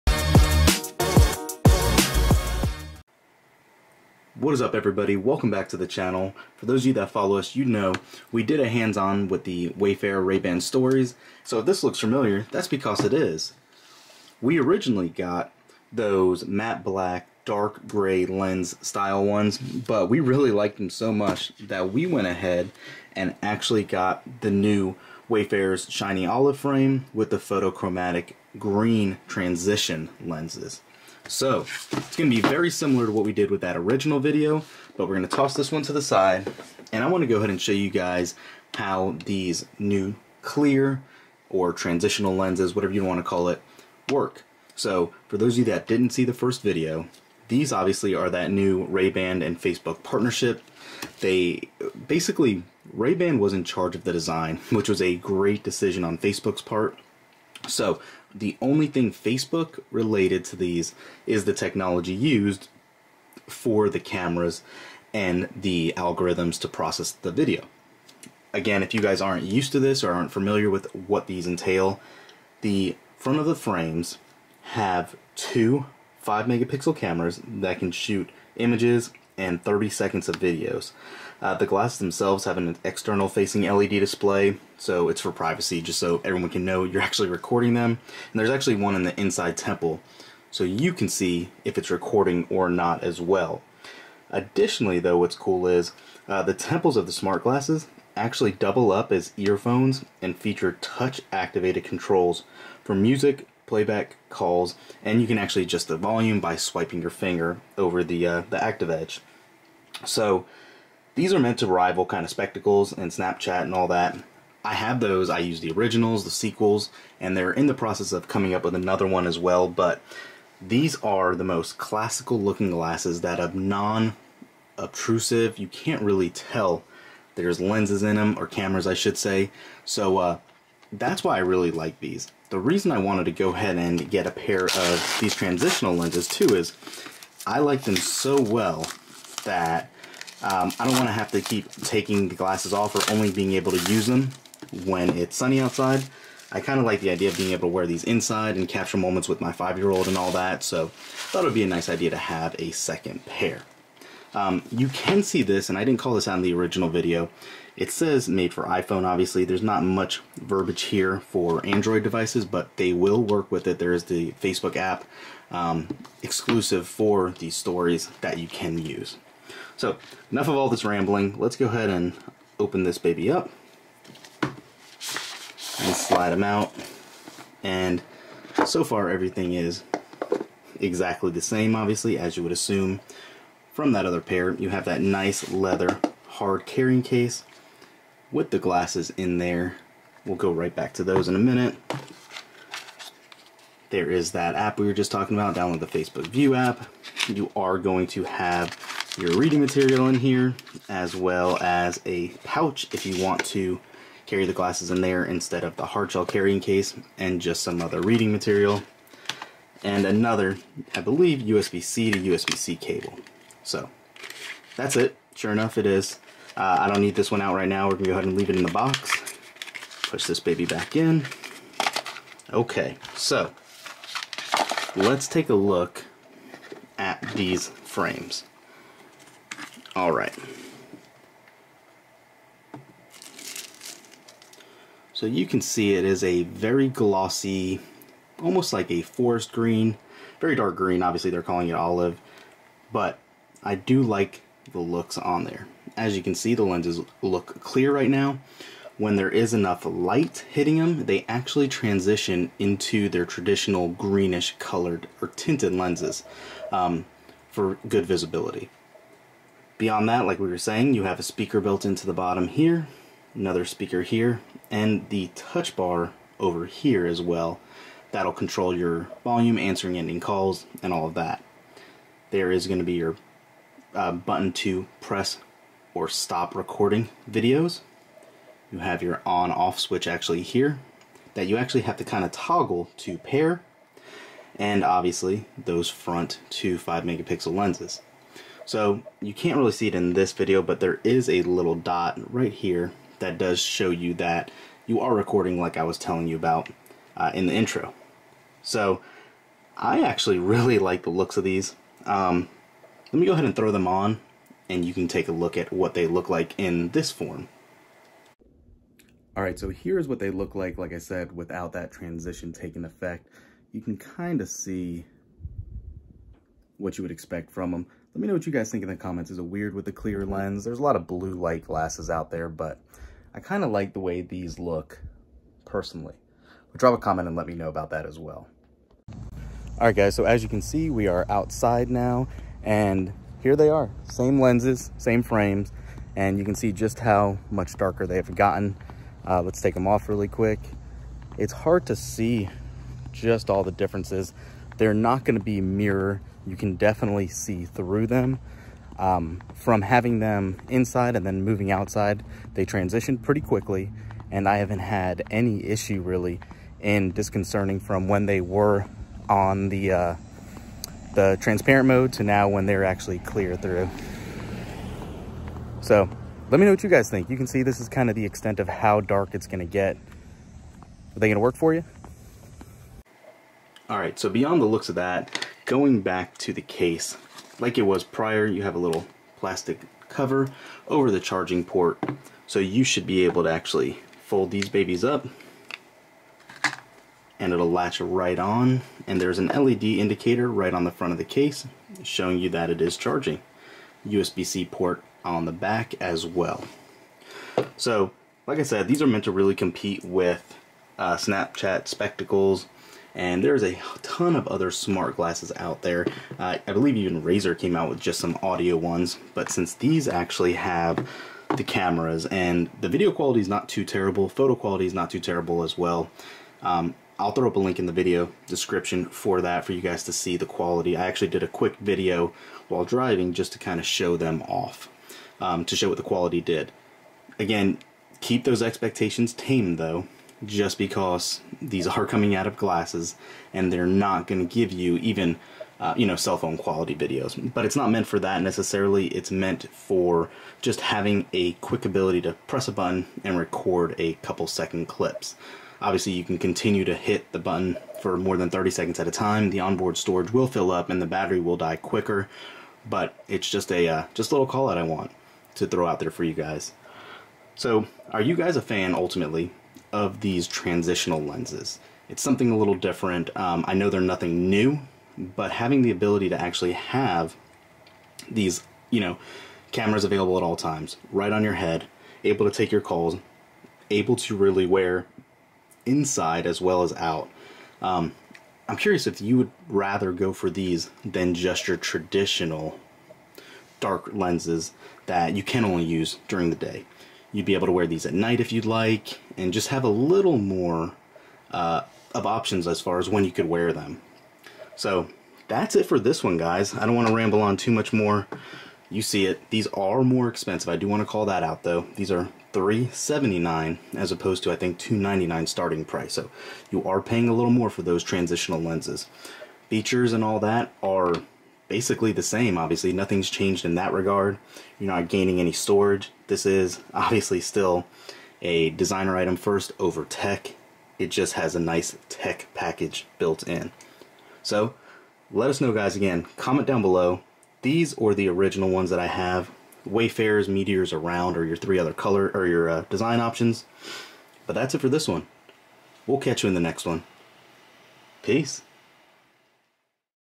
Ray-Ban Stories | Transition Lenses First Look | The Wear All Day Look
We loved the new Ray-Ban Stories so much we wanted to see if we could find more use with them by getting another pair with the transition lenses.